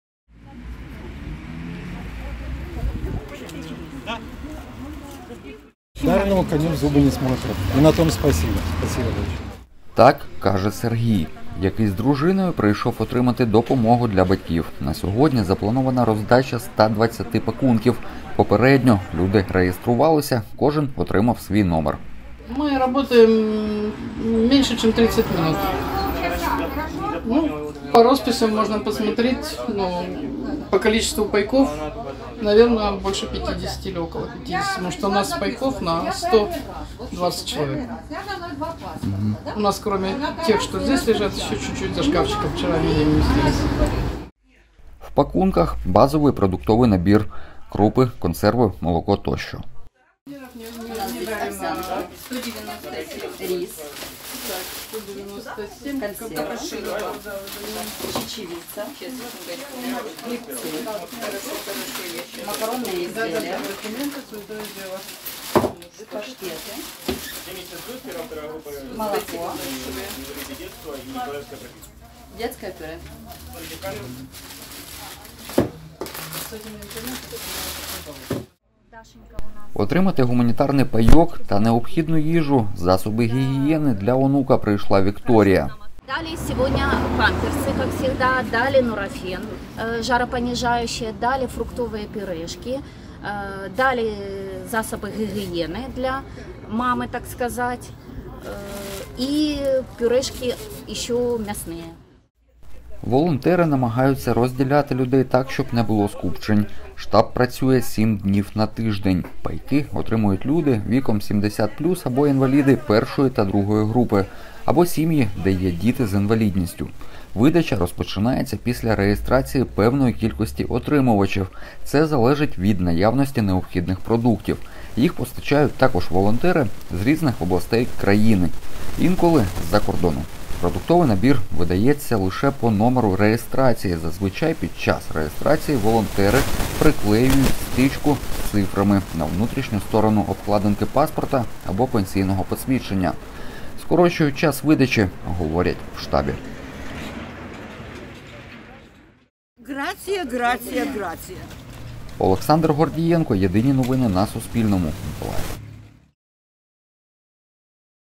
Миколаївській гуманітарний штаб: як працює та кому видають допомогу
Макаронні вироби, крупи, консерви, паштети, за потребою дитяче харчування та підгузки. У Миколаєві триває соціальна підтримка незахищених верств населення. Гуманітарний штаб у середмісті за пів години роздав понад 50 пакунків з їжею.